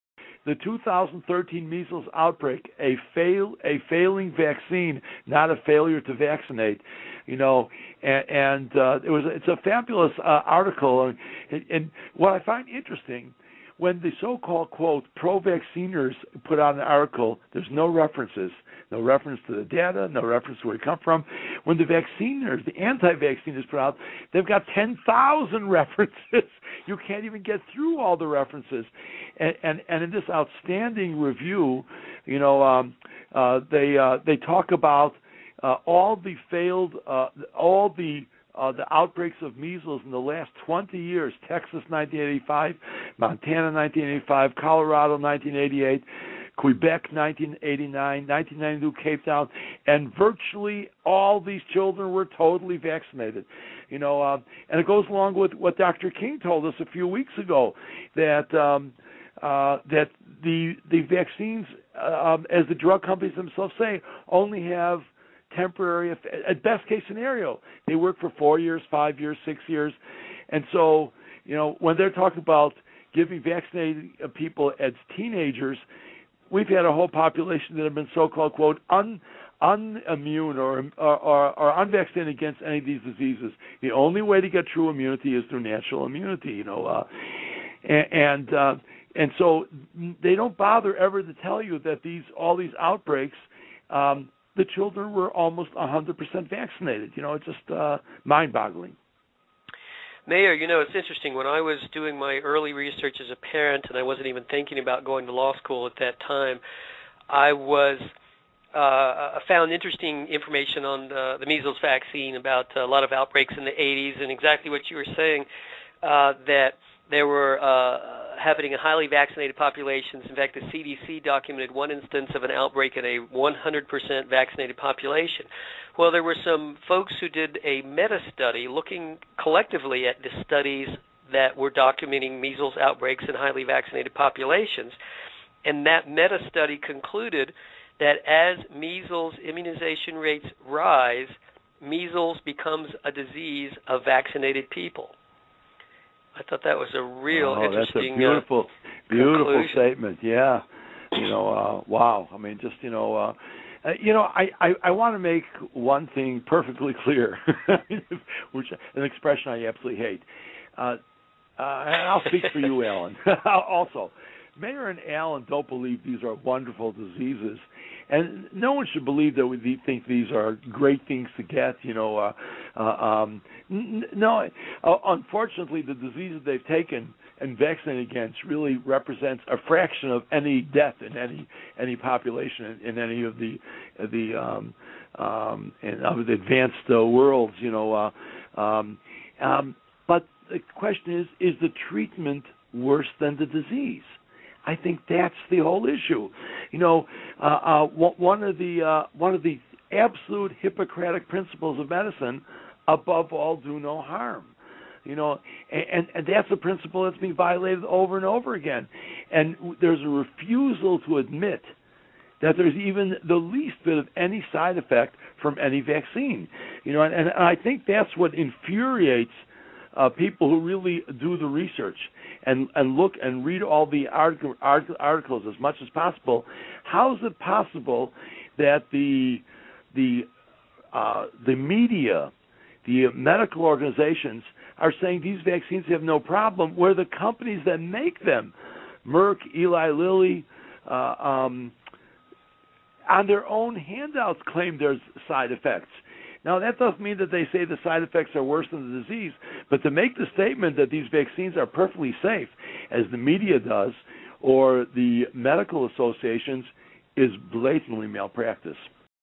Czy epidemie odry są z powodu niepowodzenia szczepionek czy z nieszczepienia
Urywek audycji radiowej prowadzonej przez dr Mayera Eisenstein'a i prawnika Alana Phillipsa.

Dr Mayer Eisenstein: „Epidemia odry w 2013r. - niepowodzenie szczepionki, a nie niepowodzenie wyszczepialności. To wspaniały artykuł. Co mi się wydaje ciekawe, jest to, że gdy tak zwane grupy pro-szczepionkowe piszą artykuł, nie ma w nich referencji. Brak odniesienia do źródła danych, nie wspomina się, skąd one pochodzą. A grupy anty-szczepionkowe mają tysiące referencji, nieraz ciężko aż przejść przez te wszystkie odniesienia.
I jest to znakomity przegląd tych wszystkich wybuchów odry w ciągu ostatnich 20 lat, począwszy od Teksasu 1985r., Montana 1995r., Colorado1988r., Quebec 1989r., Cape Town 1999r. i praktycznie wszystkie z tych dzieci były szczepione! I to idzie w parze z tym, co powiedział nam dr King kilka tygodni temu, że szczepionki, według firm farmaceutycznych, dają nam tylko chwilowy okres skuteczności. Działają one co najlepiej przez 4, 5 czy 6 lat i mówi się o dawaniu tych szczepionek ludziom, którzy jako nastolatki będą wśród tej populacji, o której się mówi, że jest nieodporna, lub nieszczepiona przeciw tym chorobom. Jedynym sposobem, aby uzyskać odporność jest naturalne przechorowanie. I nigdy nie mówią o tym, że wszystkie te wybuchy epidemii były wśród dzieci prawie w 100% szczepionych. Wiesz co, to jest niepojęte."

Alan Phillips: „Mayer, to ciekawe, wiesz, kiedy robiłem swoje pierwsze badania jako rodzic i nawet nie myślałem jeszcze wtedy o pójściu do szkoły prawniczej w tym czasie, znalazłem ciekawe informacje na temat szczepionki przeciwko odrze i wybuchami odry, w latach 80-tych, a dokładnie to, o czym mówiłeś, że zdarzały się wśród wysoko szczepionych populacji. Co więcej, CDC zgłosiło wystąpienie incydentu, gdzie to się zdarzyło wśród populacji w 100% zaszczepionych.
A było też zrobione takie mega badanie analizujące wszystkie udokumentowane przypadki wystąpienia epidemii odry w populacjach wysoce szczepionych, i w tym mega badaniu stwierdzono, że im bardziej wzrasta poziom immunizacji przeciwko odrze, tym częściej odra staje się chorobą zaszczepionych osób. Myślałem, że to bardzo ciekawe stwierdzenie."

Dr Mayer Eisenstein: „Tak, piękne ... Wiesz, chciałbym, aby jedna rzecz była zupełnie jasna. Wyrażenie, którego bardzo nie lubię... To "Mayer i Alan" nie wierzą w to, że są to wspaniałe choroby, i nikt nie powinien wierzyć, że to są wspaniałe choroby do złapania...  Niestety, te choroby, przeciw którym podjęliśmy się szczepić tak naprawdę reprezentują tylko ułamek wszelkich śmiertelności w jakiejkolwiek populacji na świecie (w rozwiniętych krajach). Ale pytanie brzmi, czy to lekarstwo nie jest gorsze od choroby? Myślę, że w tym cały problem.
Jednym z najbardziej bezwzględnych zasad Hipokratesa w medycynie jest "nie szkodzić". A to jest zasada, która zostaje w kółko naruszana. I istnieje nawet odmowa przyznania, że mogą być jakieś nawet najmniejsze efekty uboczne od jakiejkolwiek szczepionki. I myślę, że to tak irytuje ludzi, którzy sami się edukują i czytają wszystkie te artykuły tak często jak to możliwe. A jak to jest możliwe, że media i organizacje medyczne mówią, że te szczepionki nie powodują problemu, podczas gdy firmy, które je produkują jak Merck czy Eli Lilly sami mają własne materiały informacyjne, twierdząc, że są skutki uboczne. To nie znaczy, że oni powiedzą, że skutki uboczne są gorsze od choroby, ale żeby wydawać takie oświadczenia, że te szczepionki są bardzo bezpieczne, jak to się w mediach robi, lub wśród stowarzyszeń medycznych, jest rażącym błędem w sztuce lekarskiej.